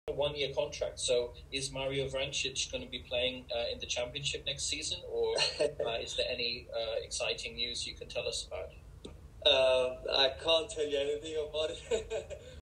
Ben McFadyean clip from interview with Mario Vrancic (Bosnia international, Stoke City FC
Interview with Mario Vrancic of Stoke City, formerly with Borussia Dortmund II, Norwich City, Paderborn 07 and Mainz 05. For the full interview, visit: Mcfadyean.podbean.com